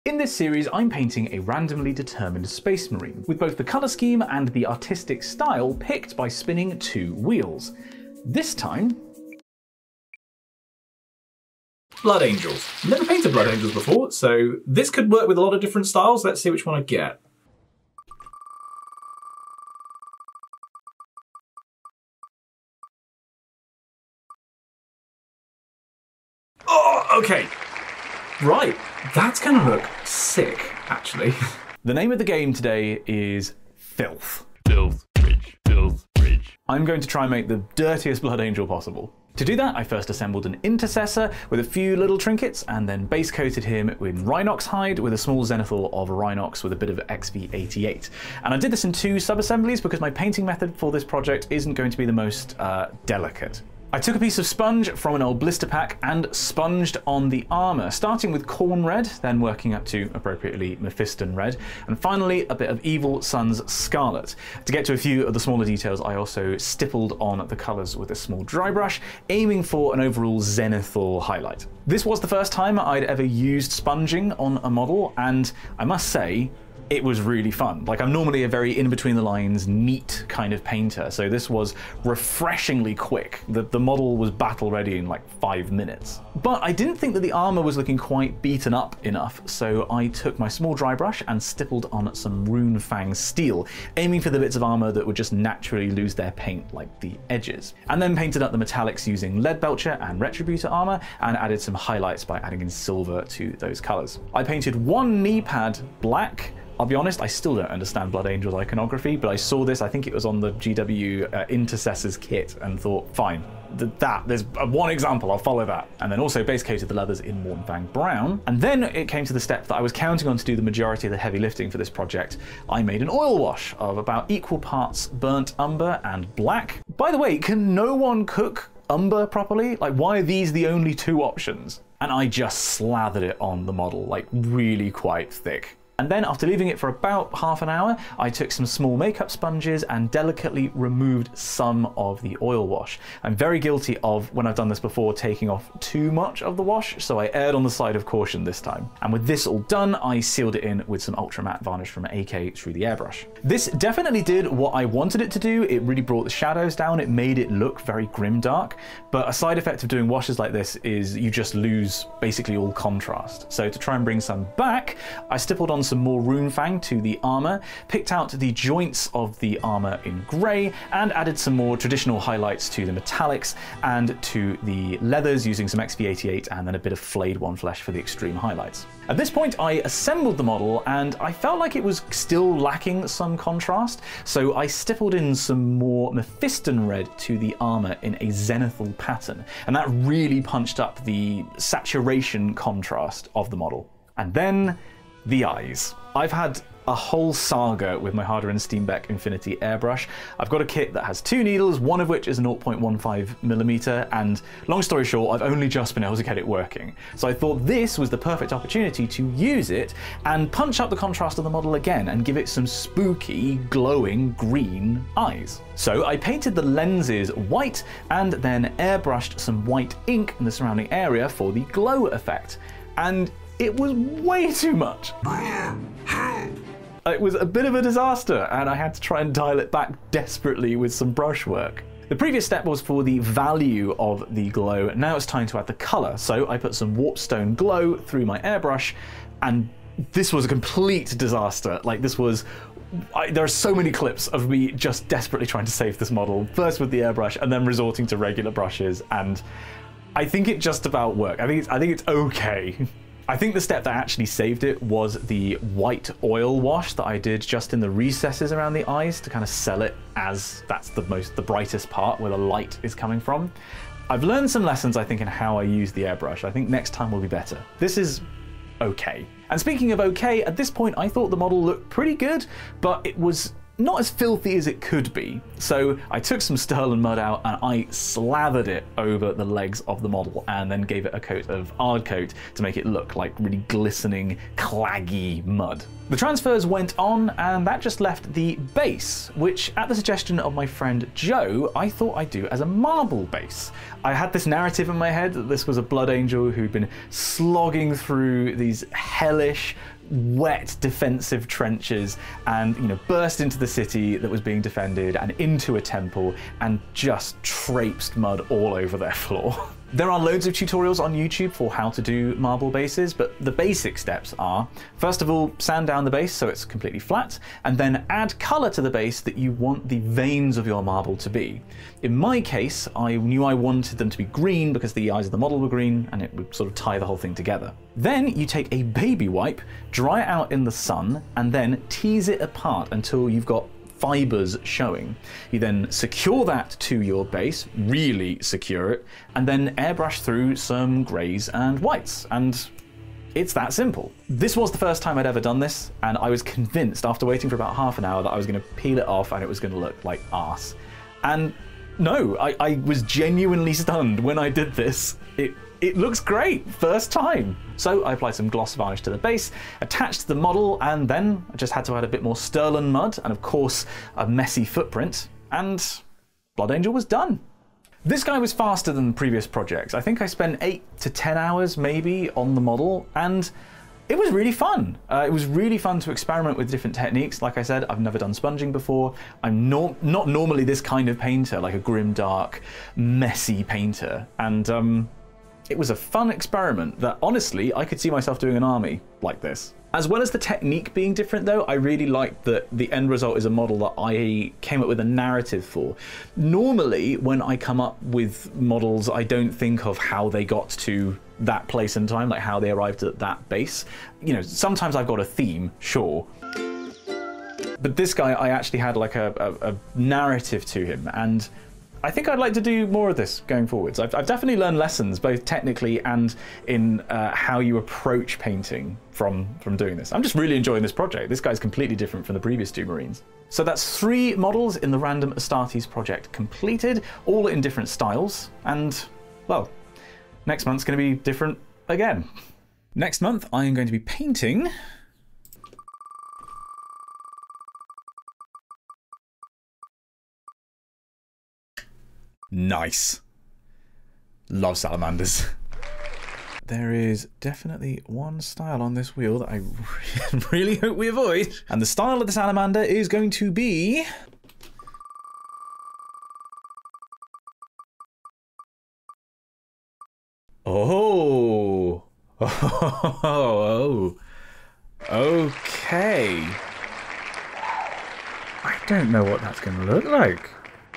Painting an ultra dirty Blood Angel | Random Astartes #3
This year I am painting one space marine intercessor a month in a randomly determined style and colour scheme. This month? Making the dirtiest Blood Angel possible. Sanguinius would be ashamed.

This was a super fun style because of how fast and messy it was. It also gave me a chance to try some marbling!